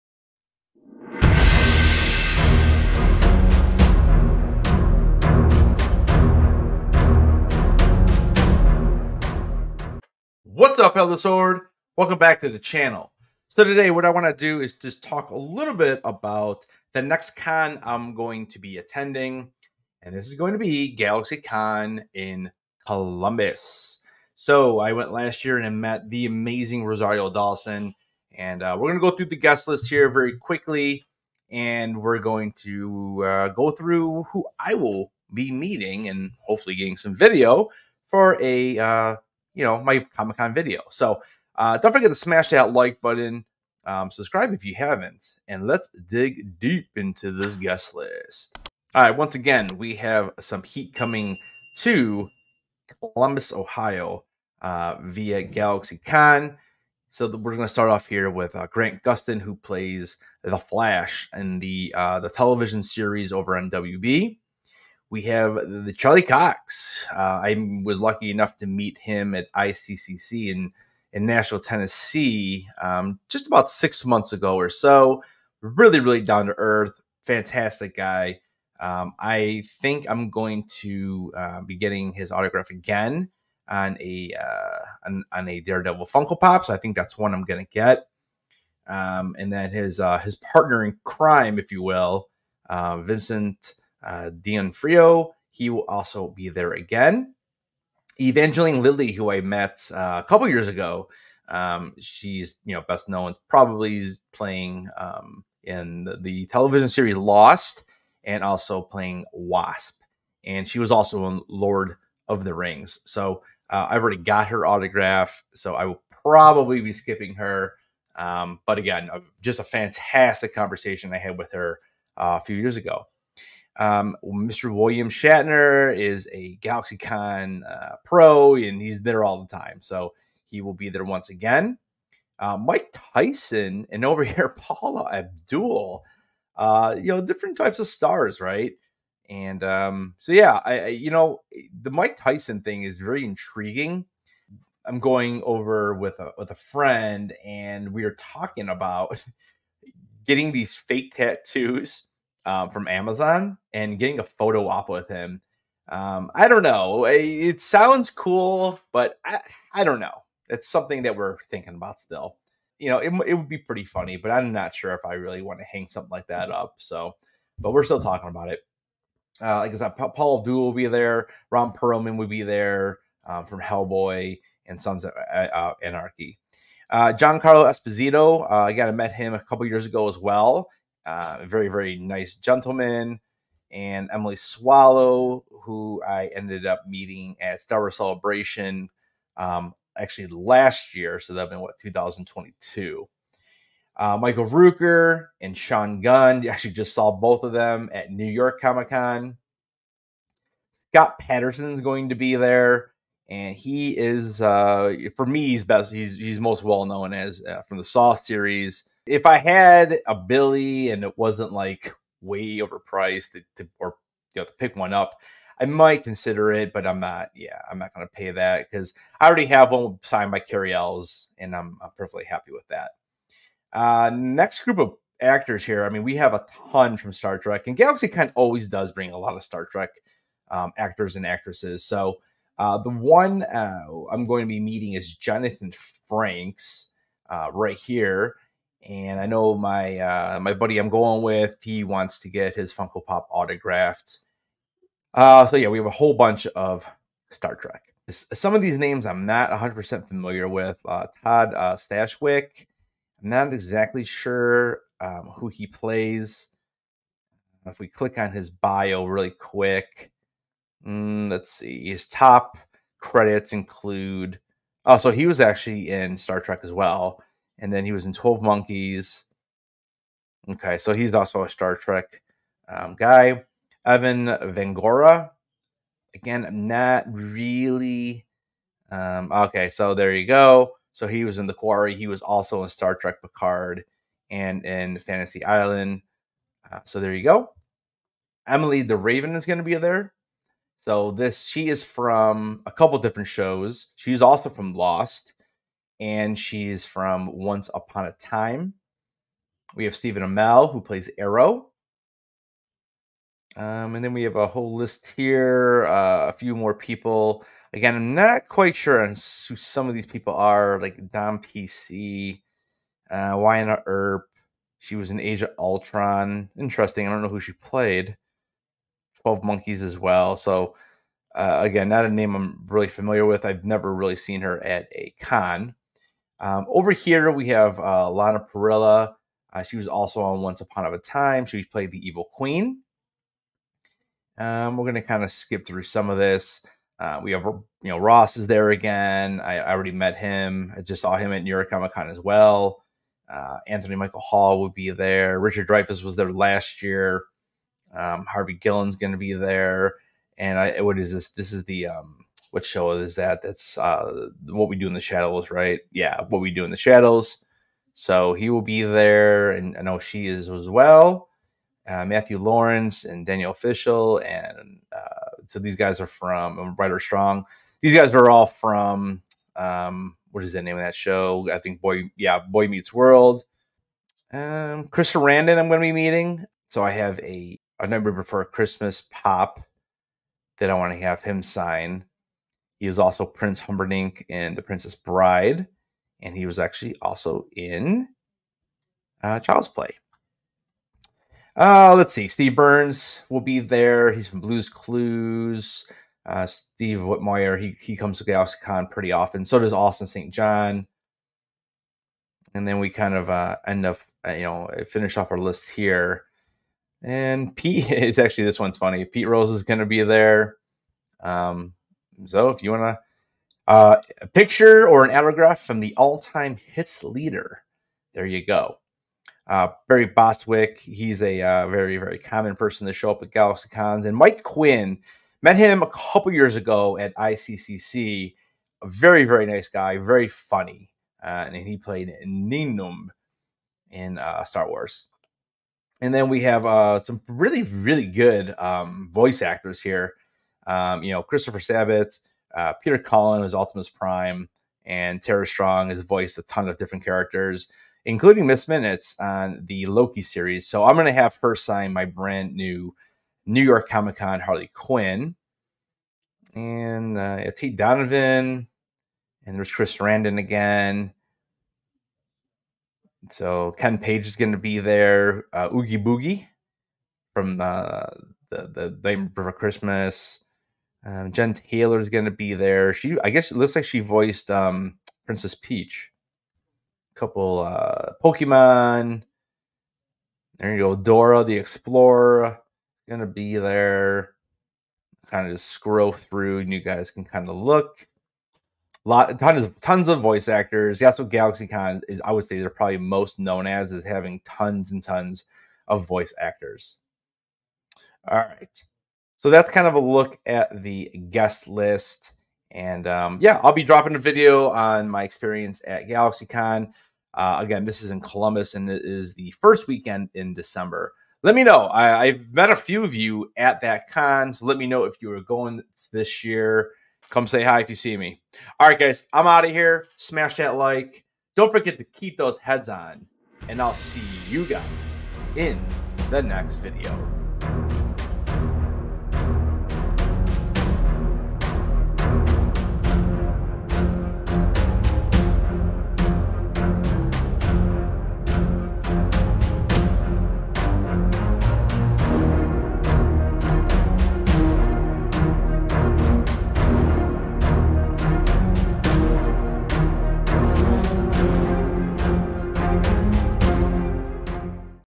NEXT CON? GALAXYCON COLUMBUS! They are bringing the heat!
In today's video I will go through the incredible guestlist for Galaxycon Columbus and talk about who I will be meeting!

Become part of the Headless Horde!